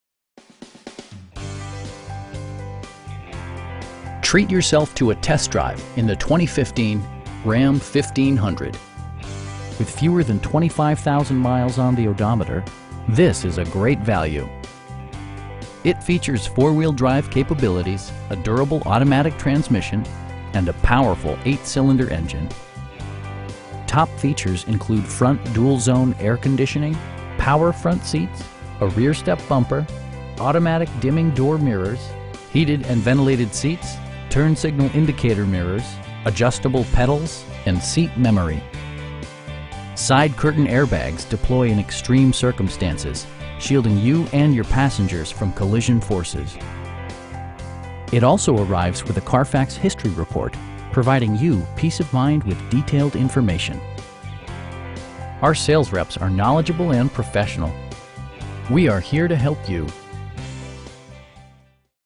2015 Ram 1500 Laramie in Roseville, MI 48066
Roseville Chrysler Dodge Jeep RAM

Treat yourself to a test drive in the 2015 Ram 1500 With fewer than 25,000 miles on the odometer, this is a great value! It features four-wheel drive capabilities, a durable automatic transmission, and a powerful 8 cylinder engine. Top features include front dual zone air conditioning, power front seats, a rear step bumper, automatic dimming door mirrors, heated and ventilated seats, turn signal indicator mirrors, adjustable pedals, and seat memory. Side curtain airbags deploy in extreme circumstances, shielding you and your passengers from collision forces. It also arrives with a Carfax history report, providing you peace of mind with detailed information. Our sales reps are knowledgeable and professional. We'd be happy to answer any questions that you may have. We are here to help you.